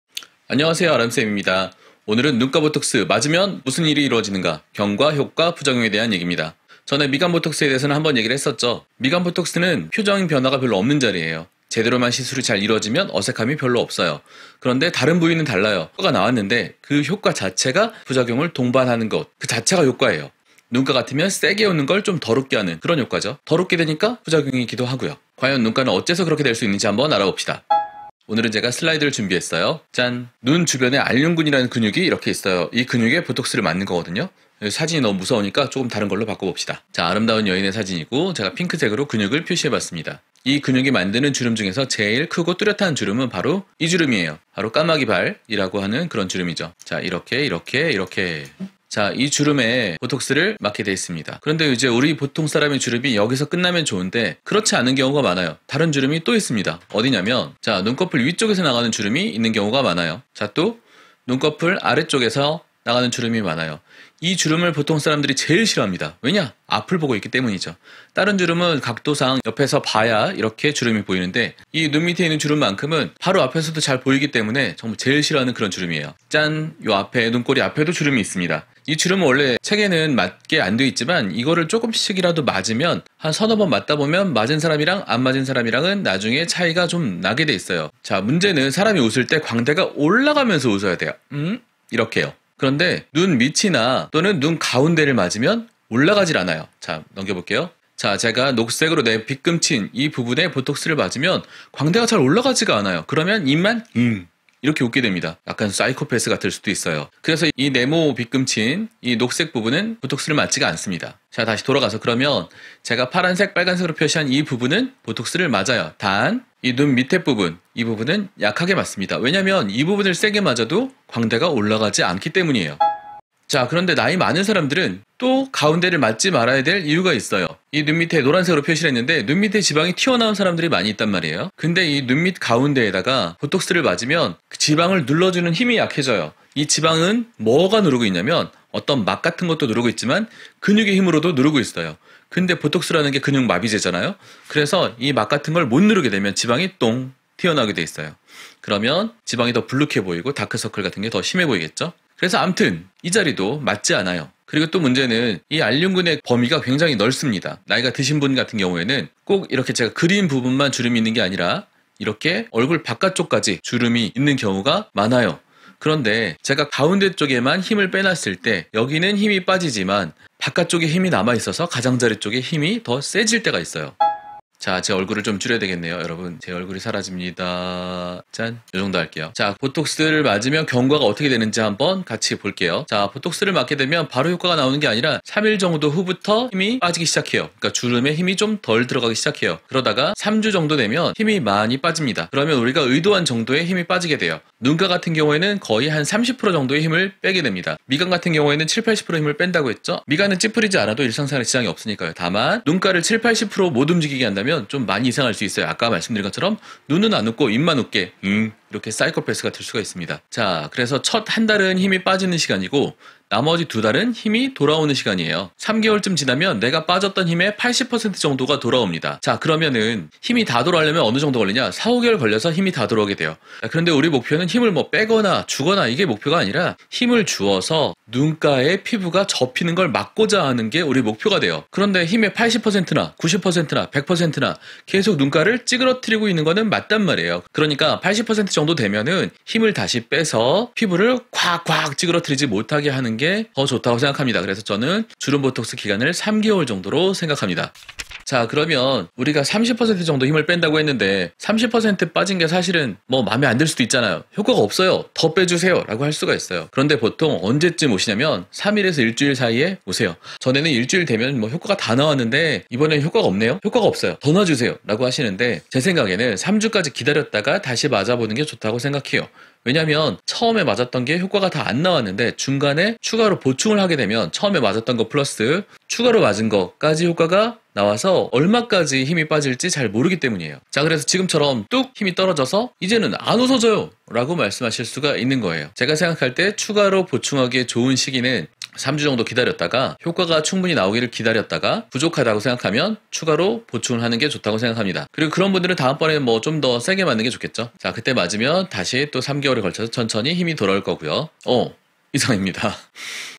눈가주름 보톡스 효과, 지속기간,어색함 무슨일이 벌어지는가?
눈가주름 보톡스 효과, 지속기간 어색함에 관한 설명입니다.
ppt와 함께는 처음인데요. 이런 스타일은 별로인지 피드백 부탁드려요.
사진에 그림을 너무 작게 그린거 같기도 합니다 ;